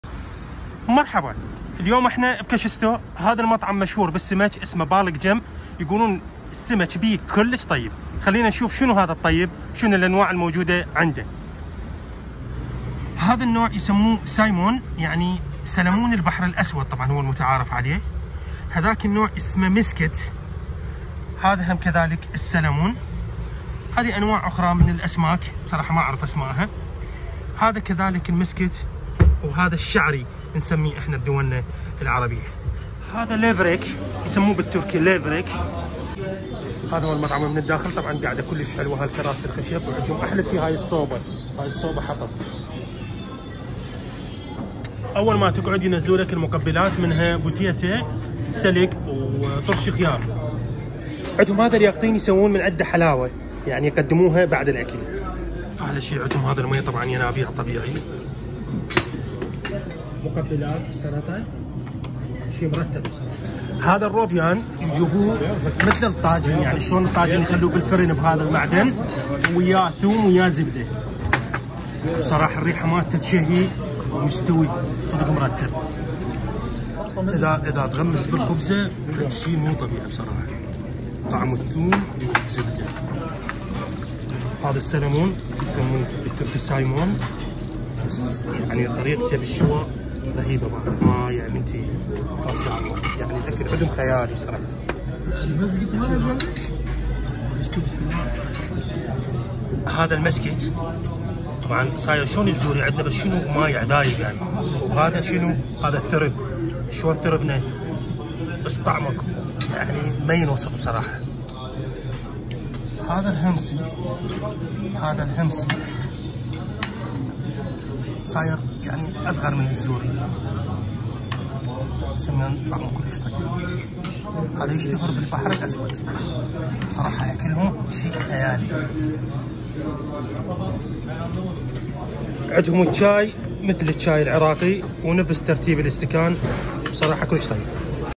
اشهر مطعم سمك في طرابزون بالك جيم عدهم ثرب خيالي وأكلهم كله طيب
تجربة انواع من سمك البحر الاسود في طرابزون